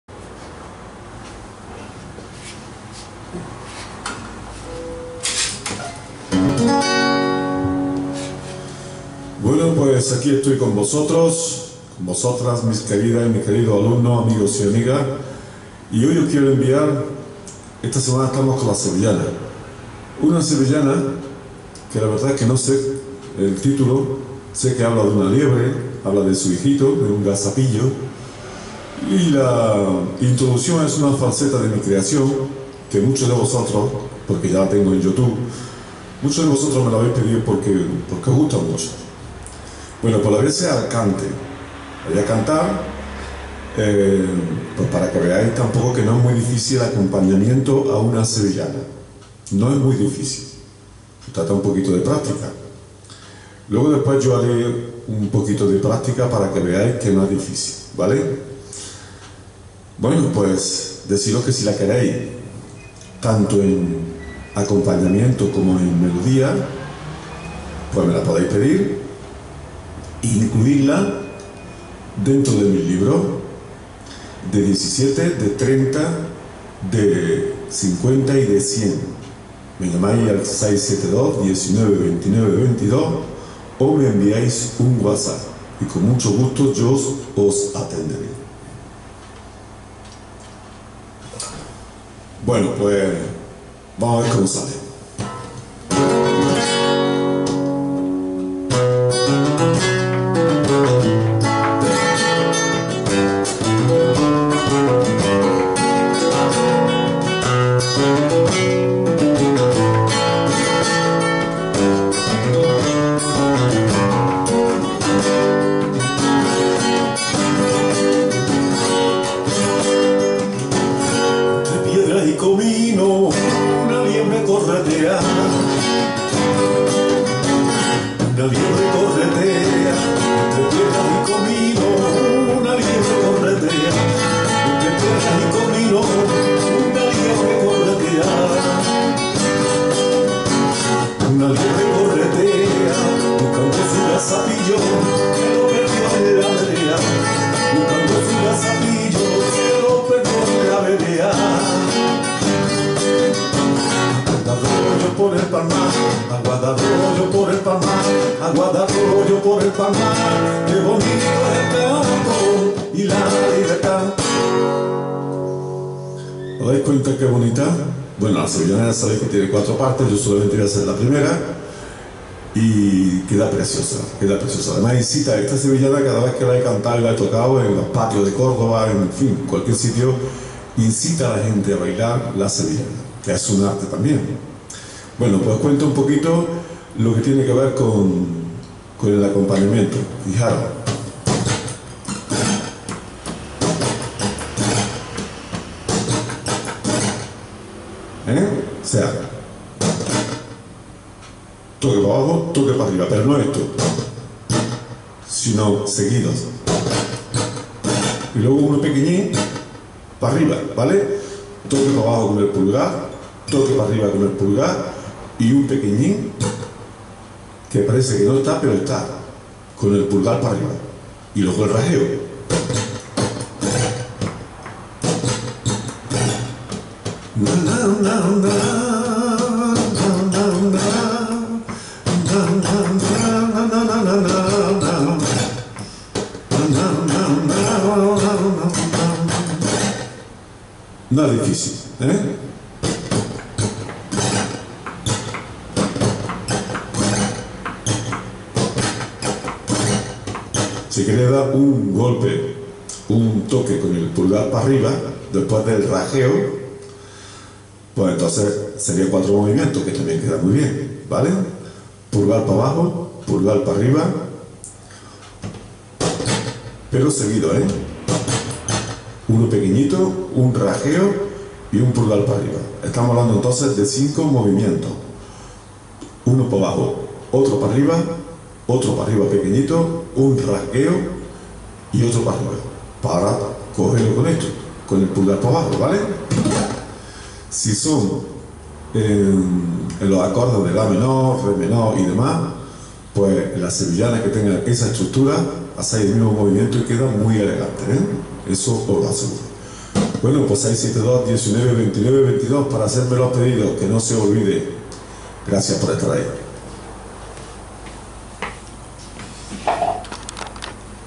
SEVILLANAS. ``ÀL CANTE Y PEQUEÑA EXPLICACION DEL ACOMPAÑAMIENTO.´´
´EL ACOMPAÑAMIENTO A UNA SEVILLANAS EN VERDAD NO ES MUY DIFICIL SI SE TIENE EL CONOCIMIENTO DE LOS TOQUES Y MOVIMIENTOS, Y EN MI METODO DE GUITARRA VIENEN MUY BIEN EXPLICADOS.   PEDIDOS AL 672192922.